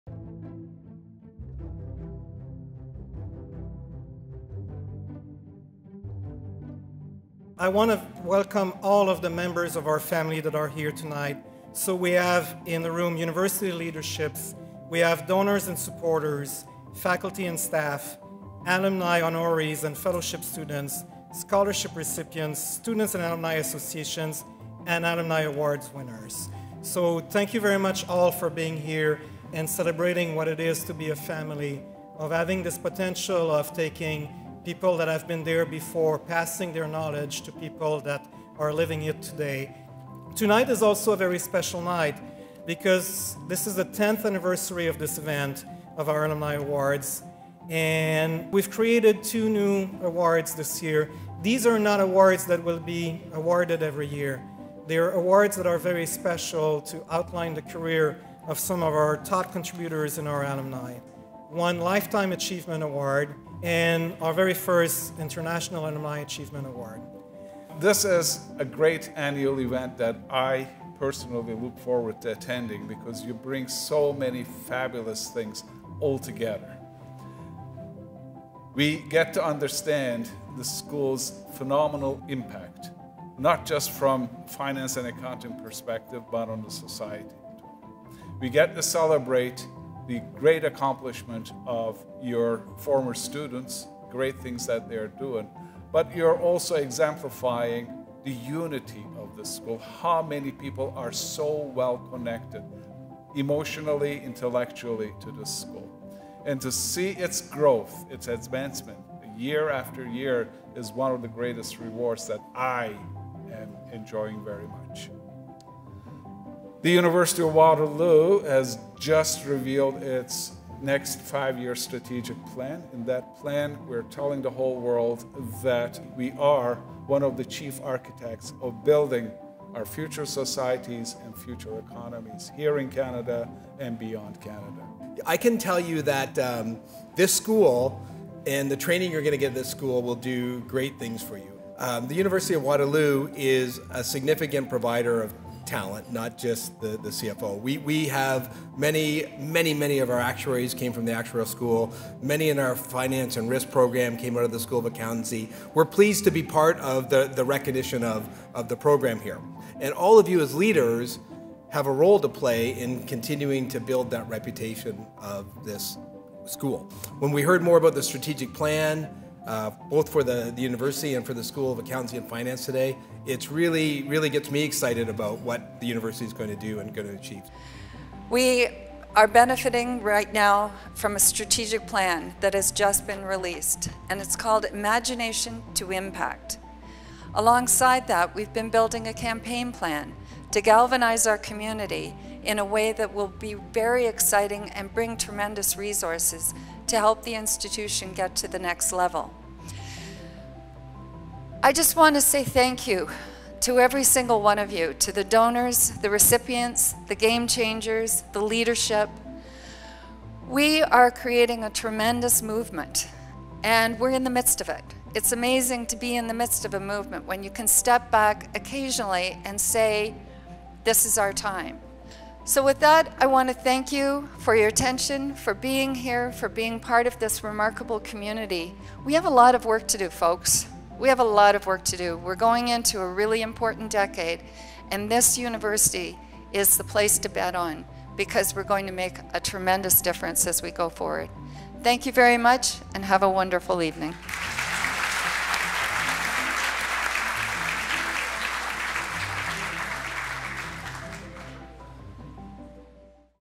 UWaterloo SAF Awards Alumni and Donors
UW SAF Awards Alumni and Donors

I want to welcome all of the members of our family that are here tonight. We have, in the room, university leaderships, we have donors and supporters, faculty and staff, alumni honorees and fellowship students, scholarship recipients, student alumni associations, and alumni awards winners. Thank you very much all for being here and celebrating what it is to be a family of having this potential of taking people who have been there before, passing their knowledge to people that are living it today. Tonight is also a very special night because this is the 10th anniversary of this event of our alumni awards and we’ve created two new awards this year. These are not awards that will be awarded every year. They are awards that are very special to outline the career of some of our top contributors and our alumni. One Lifetime Achievement Award, and our very first International Alumni Achievement Award.
This is a great annual event that I personally look forward to attending because you bring so many fabulous things all together. We get to understand the school’s phenomenal impact, not just from finance and accounting perspective, but on the society. We get to celebrate the great accomplishment of your former students, the great things that they’re doing, but you’re also exemplifying the unity of the school – how many people are so connected, emotionally, intellectually to the school. To see its growth, its advancement, year after year, is one of the greatest rewards that I am enjoying very much. The University of Waterloo has just revealed its next five year strategic plan. We’re telling the whole world we are one of the chief architects of building our future societies and future economies, here in Canada and beyond Canada.
I can tell you that this school, and the training you’re going to get at this school, will do great things for you. The University of Waterloo is a significant provider of talent, not just the CFO. We have many, many, many of our actuaries come from the actuarial school. Many from our finance and risk program came from the School of Accountancy. We’re pleased to be part of the recognition of the program here, and all of you as leaders have a role to play in continuing to build that reputation of this school. When we heard more about the strategic plan, both for the university and for the School of Accountancy and Finance today, it really gets me excited about what the university is going to do and achieve.
We are benefiting right now from a strategic plan that has just been released and it’s called, “Imagination to Impact”. Alongside that, we’ve been building a campaign plan to galvanize our community in a way that will be very exciting and bring tremendous resources to help the institution get to the next level. I just want to say thank you to every single one of you. To the donors, to the recipients, the gamechangers, the leadership, we are creating a tremendous movement and we’re in the midst of it. It’s amazing to be in the midst of a movement when you can step back occasionally and say, “this is our time!”. So with that, I want to thank you for your attention, for being here, for being apart of this remarkable community. We have a lot of work to do, folks. We have a lot of work to do. We’re going in to a really important decade, and this university is the place to bet on because we’re going to make a tremendous difference as we go forth. Thank you very much, and have a wonderful evening.